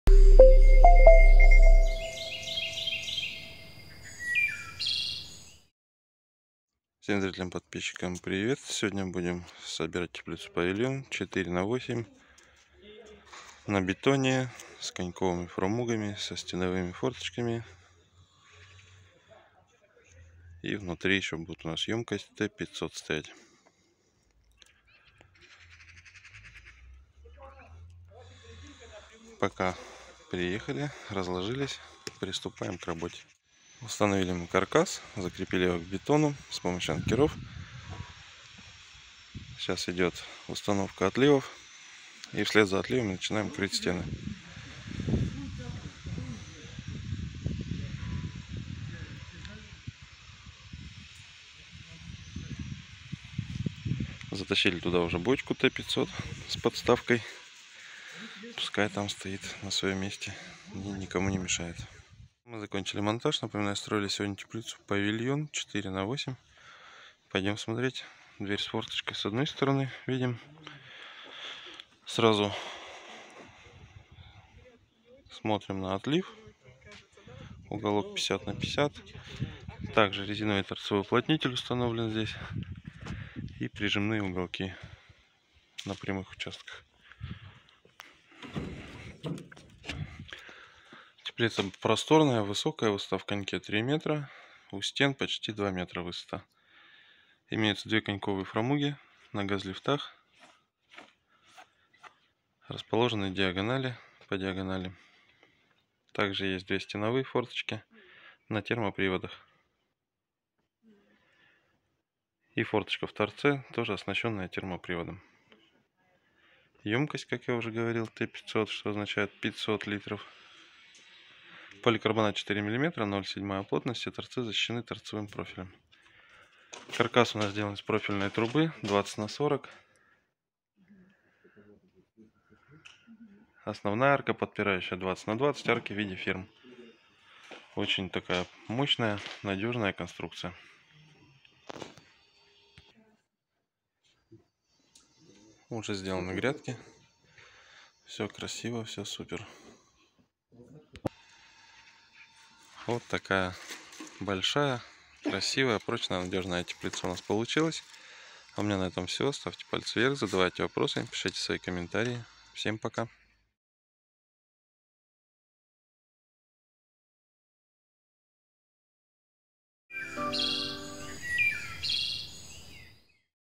ПАВИЛЬОН 4 МЕТРА ШИРИНОЙ. АРОЧНАЯ КРЫША И ВЫСОКИЕ СТЕНЫ!
В нашей компании есть модель, удовлетворяющая всем вашим запросам. Это «Павильон 4М»
«Павильон» это высокая 3 метра, теплица с прямой стенкой 1,95 метра. Большая, просторная и удобная. Размеры данной модели, ширина 4 метра и длина на ваш выбор, также мы можем сделать теплицу по индивидуальному заказу.
Каркас сделан из профильной оцинкованной трубы с порошково-полимерной покраской. Для усиления каркаса сделана двойная арка и диагональные прогоны. Сверху поликарбонат высокой плотности,  толщиной от 4 мм. Соты поликарбоната защищены от грязи и влаги перфолентой и торцевым профилем. В теплице можно установить глухую перегородку из поликарбоната для выращивания различных видов растений.
Для проветривания можно установить торцевые  двери с форточкой, стеновые форточки  и коньковые  фрамуги с термоприводом. Такое сочетание создает идеальный микроклимат и проветривание для ваших растений.

Для удобства подвязки растений, можно установить систему подвязки. А если дополнительно установить капельный автополив, то вам остаётся только урожай собирать! Вот такой «Павильон» получился. Хотите такую теплицу на вашем участке? Пишите, звоните и приезжайте!
За расчетом и консультацией
🏡 Екатеринбург ул Климовская 23
🖥 ural-usadba.ru